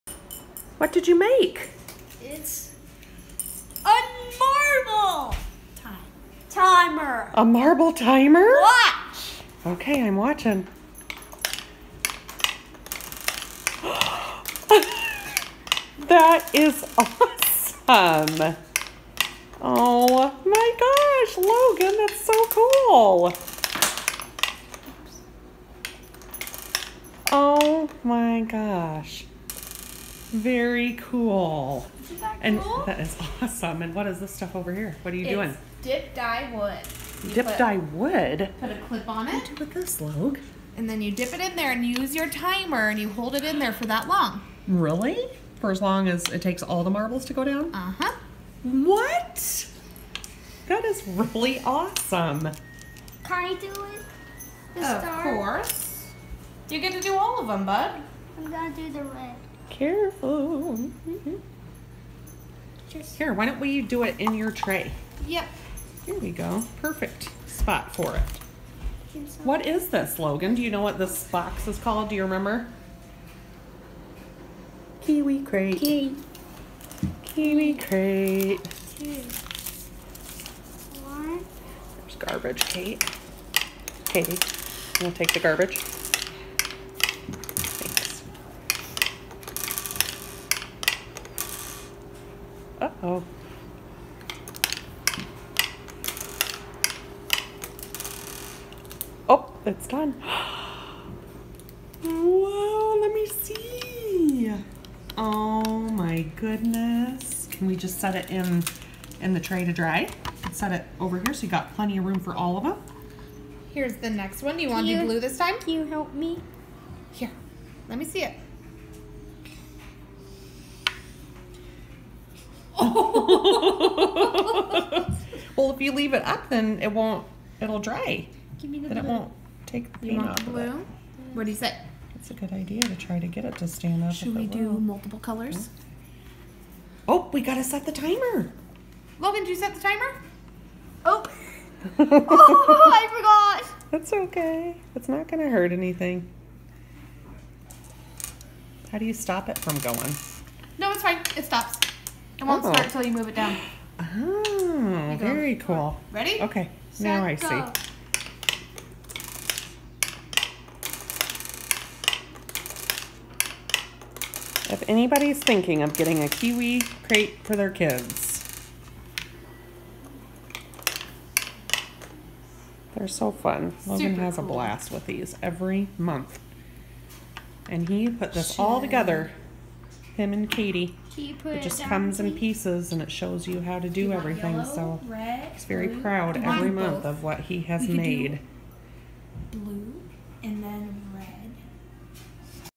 Kiwi Crate Marble Timer
We’ve really enjoyed our Kiwi Crate subscription. Our 7 year old with ASD has never been disappointed with it.